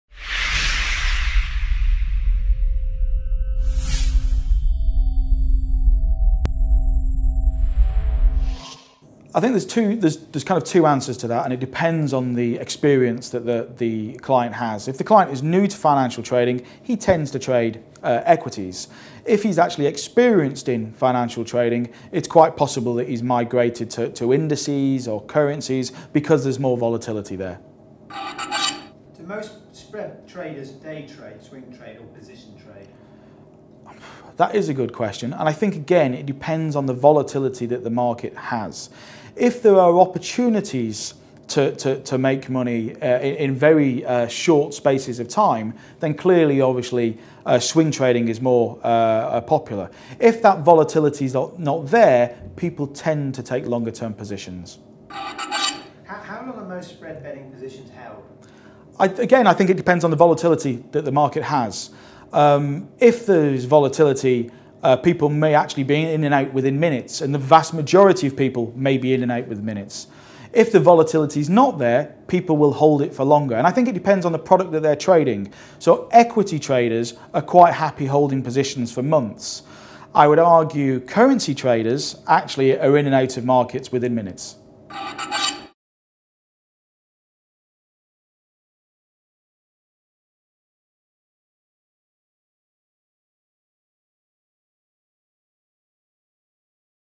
Do most spread traders day trade, swing trade or position trade?
More popular markets.   Daniel Moczulski of Star Financial Systems comments.  Do most spread traders day trade, swing trade or position trade?  How long are most spread betting positions held?  This depends on clients experience - if a trader is new to trading he's more inclined to trade stocks and shares; with experience he's more likely to migrate to indices and forex trading because there is more volatility there.

Equity traders are quite happy holding positions for months, currency traders are more inclined to trade in and out of positions within minutes.